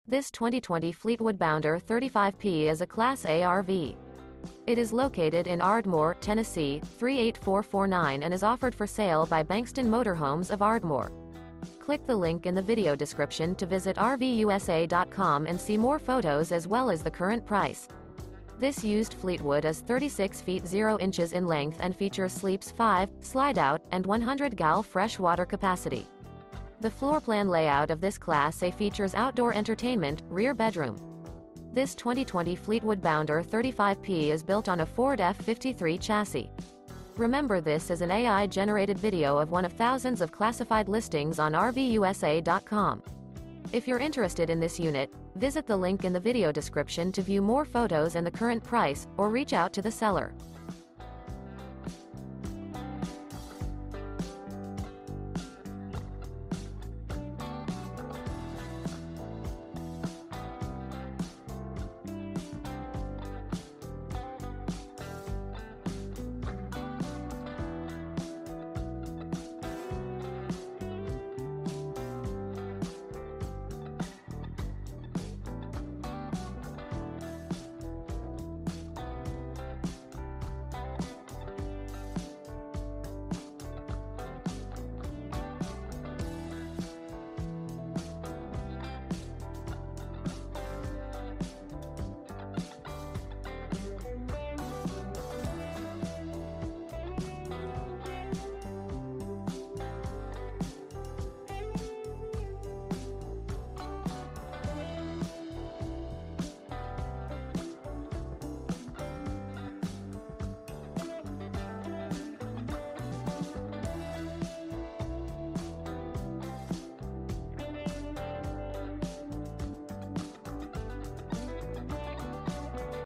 Eye-catching 2020 Fleetwood Bounder 35P Class A RV For Sale in Ardmore, TN | RVUSA.com
This 2020 Fleetwood Bounder 35P is a Class A RV. It is located in Ardmore, TN 38449 and is offered for sale by Bankston Motor Homes of Ardmore. This Used Fleetwood Is 36' 0" in length and features sleeps 5, Slideout, and 100 gal fresh water capacity. The floorplan layout of this Class A features Outdoor Entertainment, Rear Bedroom. This 2020 Fleetwood Bounder 35P is built on a Ford F-53 chassis.